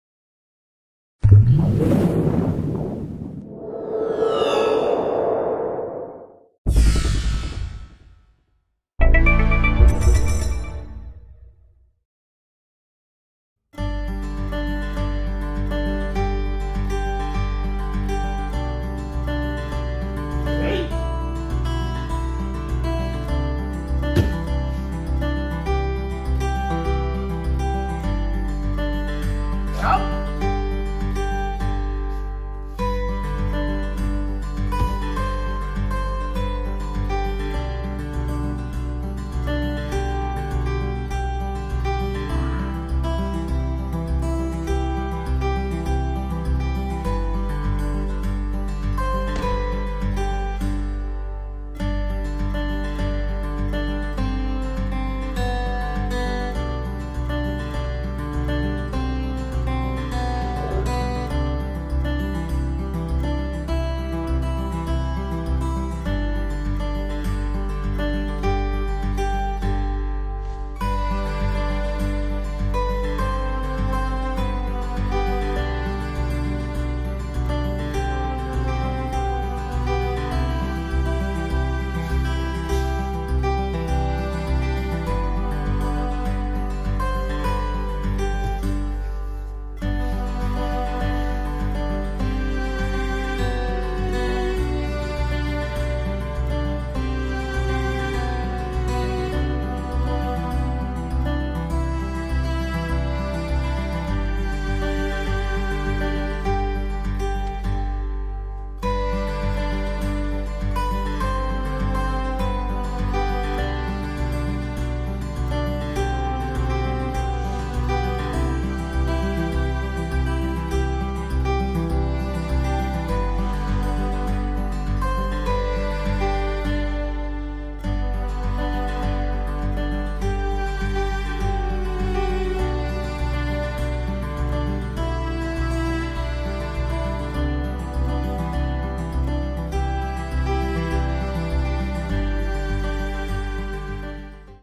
Hijidori DAI GOKYO (ura) - Marco Rubatto
Forma di base (kihon) e fluida (ki no nagare) di "presa al gomito e applicazione del 5º principio" (hijidori dai gokyo), forma posteriore (ura)

Basic form (kihon) and fluid form (ki no nagare) of "elbow grip and application of the 5th principle" (hijidori dai gokyo), backward form (ura)